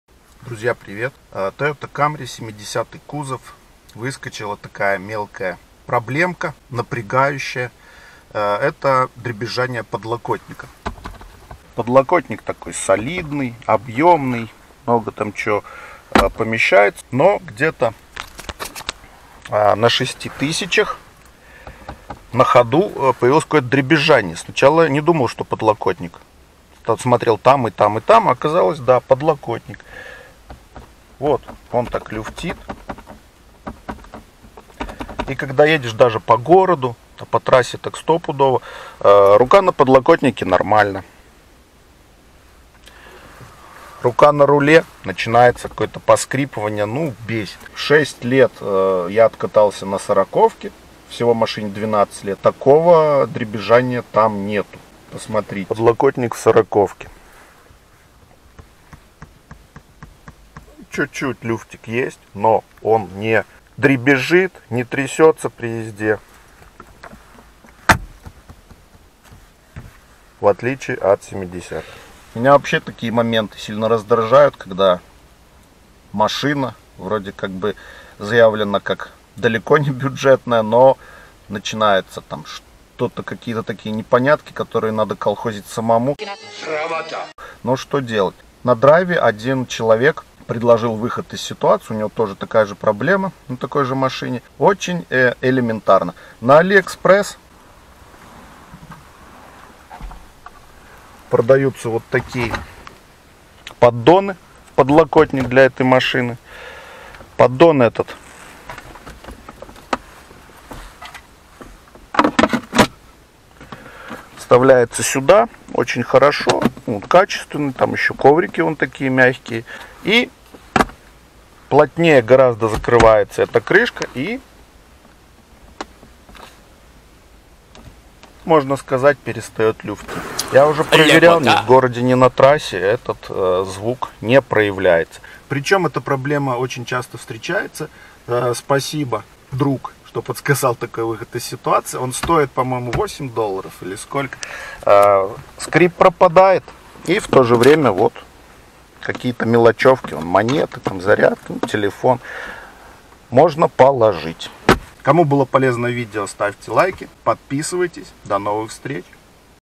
Дребезжание подлокотника Тойота Камри 70
Чуть менее, чем на 6000км пробега появился раздражающий скрип во время движения сначала по трассе, а затем и по городу. Выяснилось, что скрипит крышка подлокотника. В интернете подсказали выход из ситуации (оказывается, что это не единичный случай!!!). Какой - смотрите в этом видео...

Зарегистрируйтесь по моей ссылке на сайте AirBnB и получите скидку на первое бронирование жилья для вашего путешествия